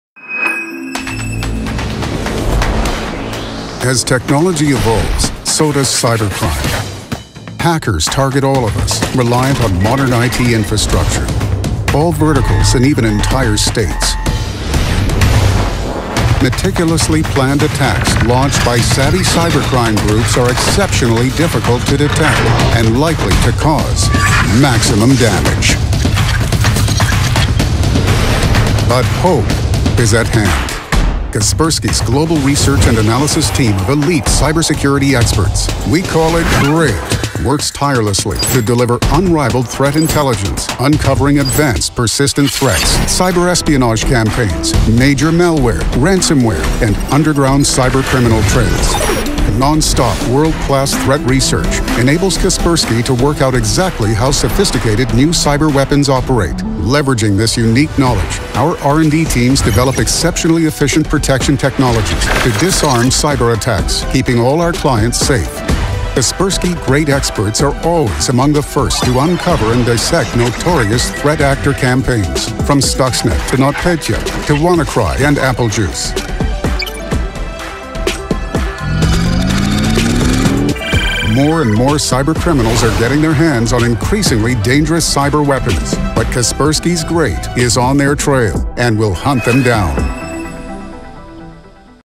Kaspersky Global Research and Analysis Team
Kaspersky Threat Intelligence, driven by our Global Research and Analysis Team of world-leading researchers with an unrivalled track record in spotting new threats, harnesses petabytes of proprietary malware data to deliver context-rich, immediately actionable intelligence integrated seamlessly into existing security controls.